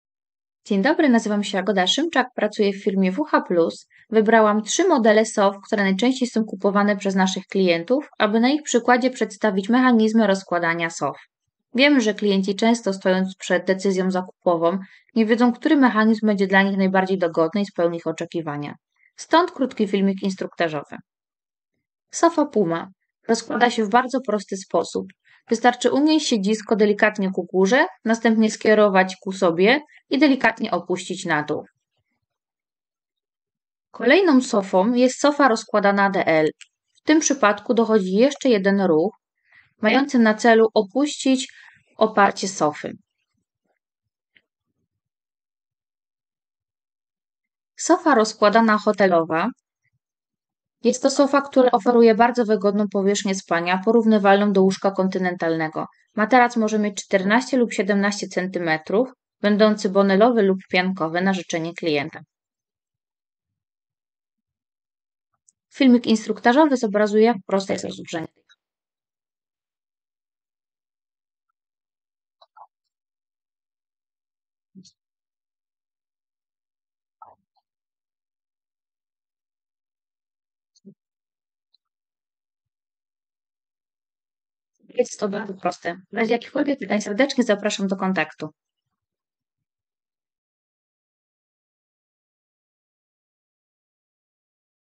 Mechanizmy rozkładania sof hotelowych
W filmie Jagoda Szymczak, specjalista e-commerce w firmie WH Plus, przedstawia różne mechanizmy rozkładania i składania sof hotelowych.